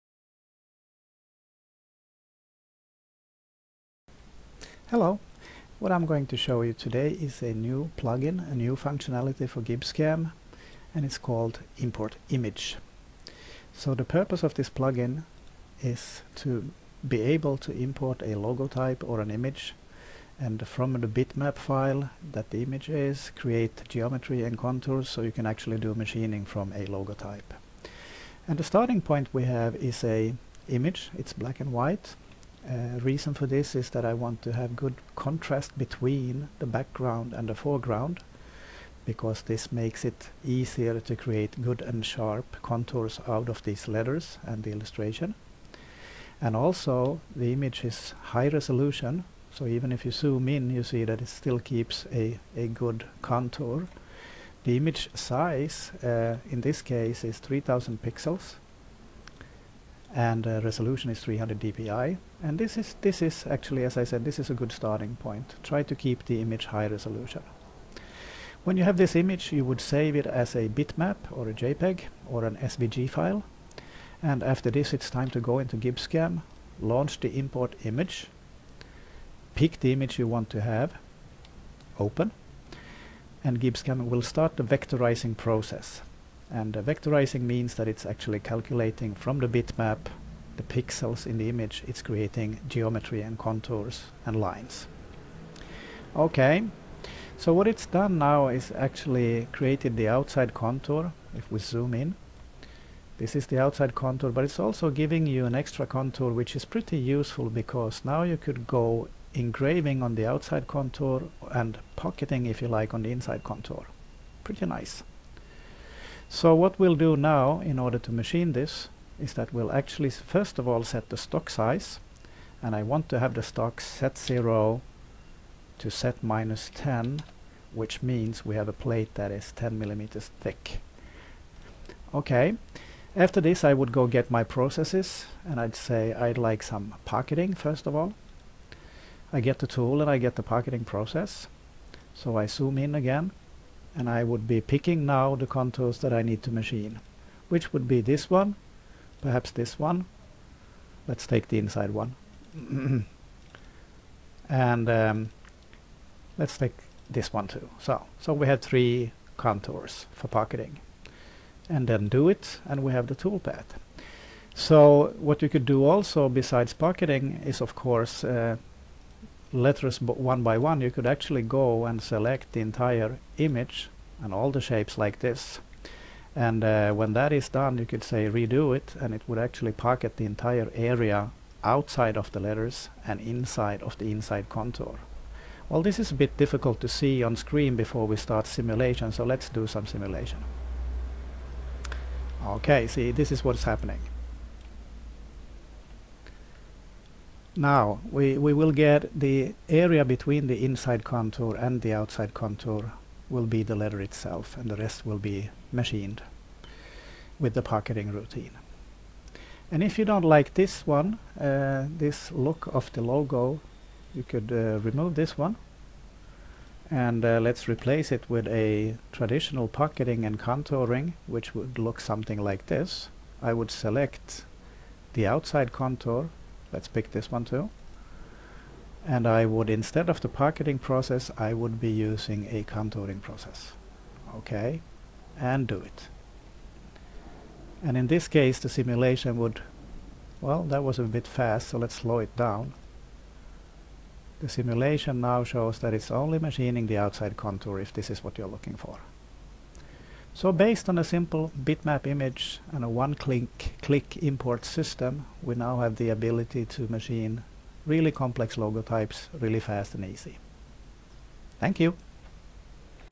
GibbsCAM - Raster-To-Vector plugin
@GibbsCAM "Raster-To-Vector" PlugIn from CAM Solutions - a fast and easy-to-use module for importing logotypes, illustrations and images for machining. Automatic creation of geometry from BMP, JPG and SVG. Support Pocketmilling, Contouring and Engraving.